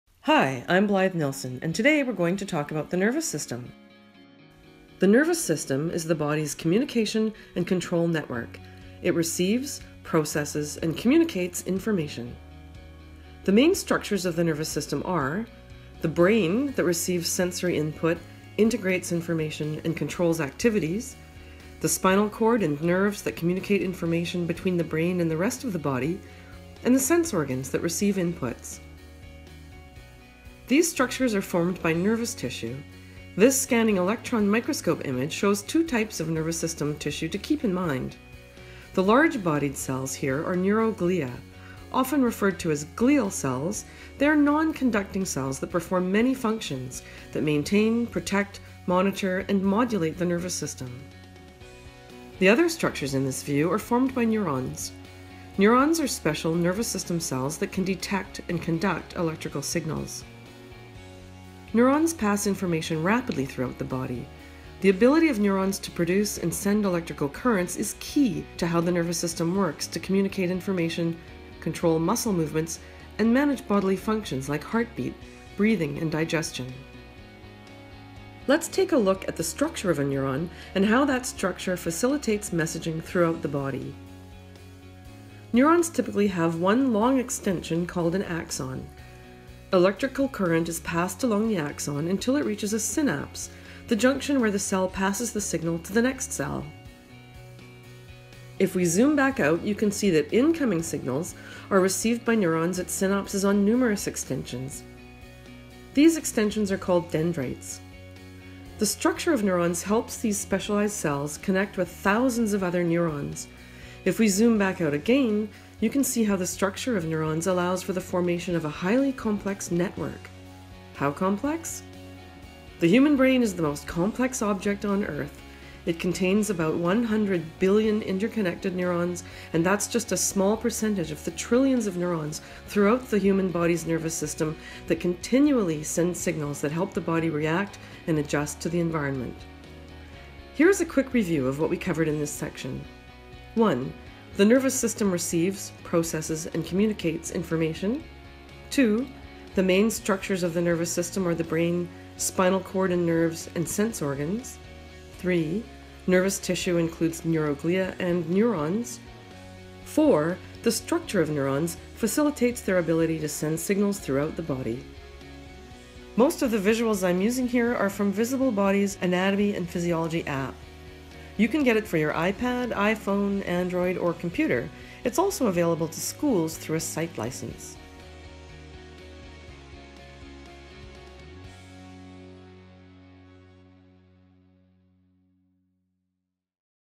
Nervous System Lesson 1: An Introduction to Key Structures of the Nervous System
Take a 3-minute dive into the nervous system with Professor Nilson of the University of British Columbia. In this video, you will learn: how the nervous system receives, processes, and communicates information, the main structures of the nervous system (brain, spinal cord, nerves, and sense organs), and what nervous tissue is (neuroglia and neurons).

Visible Body produces innovative, interactive 3D human anatomy and physiology content and applications. If you love the human body, you'll love us!